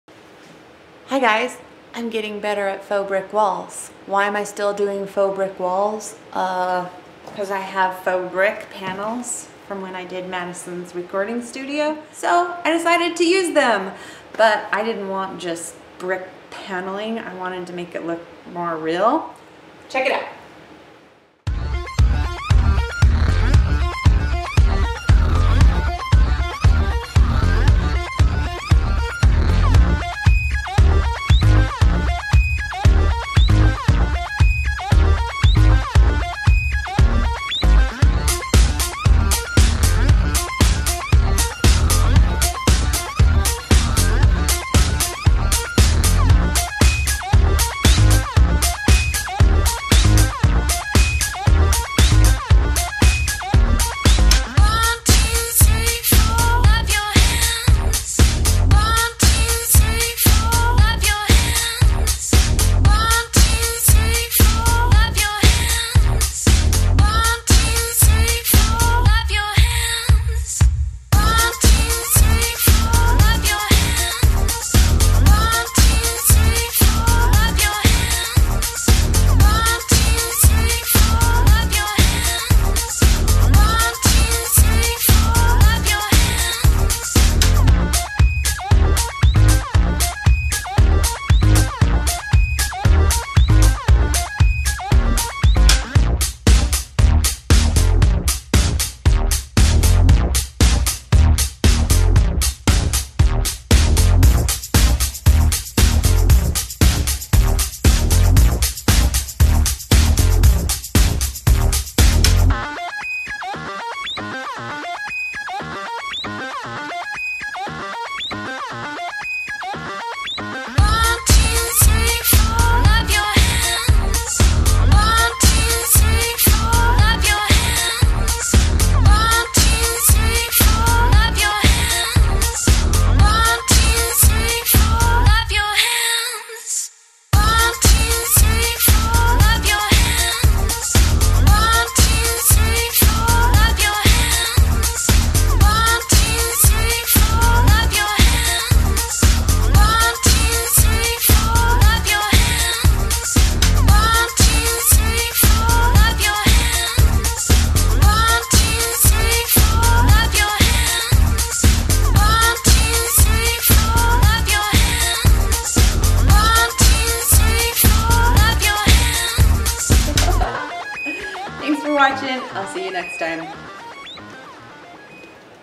Faux Brick and Chipped Plaster Wall
I created a second brick and plaster wall in my Studio.  I used a different approach with this one.  I actually cut away the drywall and installed brick paneling in the cut-outs.  I used common joint compound for the "plaster".  1 because I wanted it to dry slowly so I'd have enough time to form it the way I wanted it.  It's an artistic process.  Hot mud would have dried too fast and rushed the process.